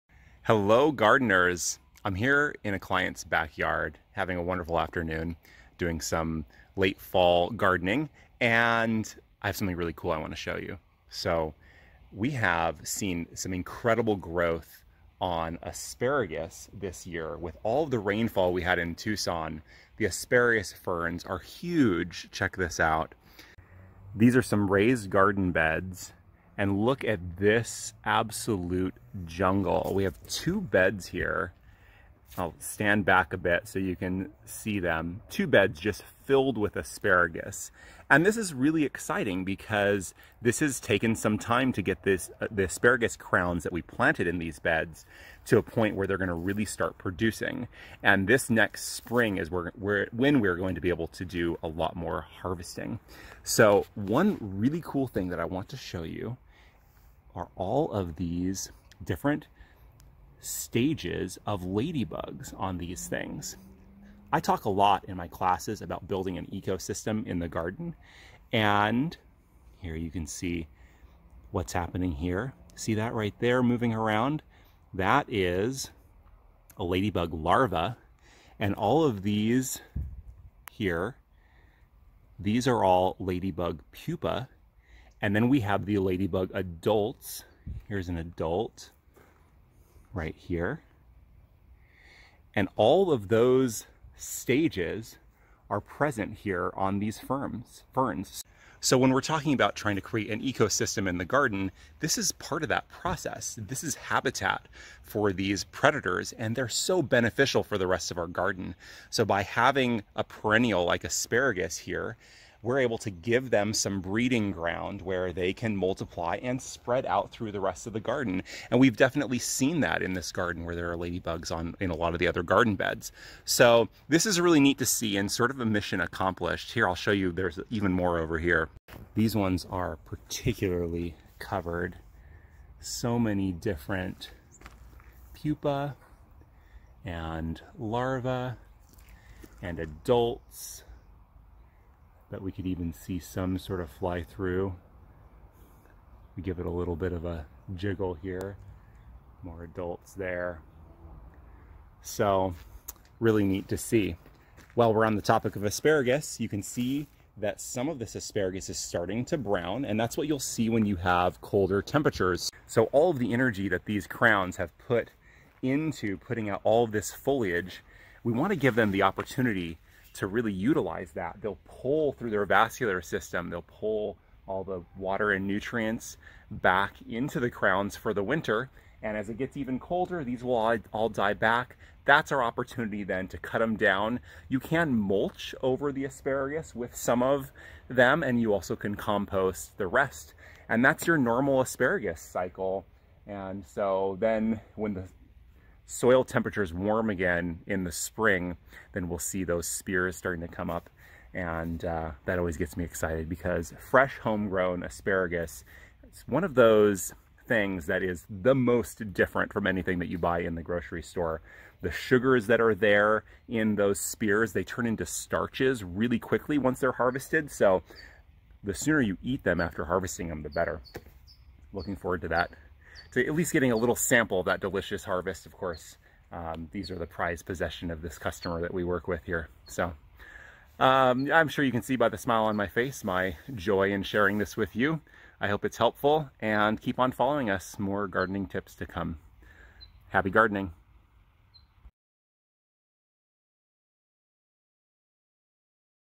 ASPARAGUS: The perfect ladybug habitat. Attract more beneficial insects for garden success!
Are you wondering what you can do to attract more beneficial insects such as ladybugs to your garden? Do you also want to grow the best asparagus you've ever eaten? In this video, expert gardener Michael Ismail covers both! Check out how these asparagus ferns have become an incredible ladybug breeding ground. These ladybugs are beneficial insects for garden success. Ladybugs eating aphids, and reproducing in this ladybug paradise are also spreading throughout the rest of the garden and keeping aphids off the rest of our plants.  You will also receive some tips on asparagus fern care such as what to do with the ferns as they start to die back in the winter. Proper asparagus fern plant care can lead to better harvests in the spring.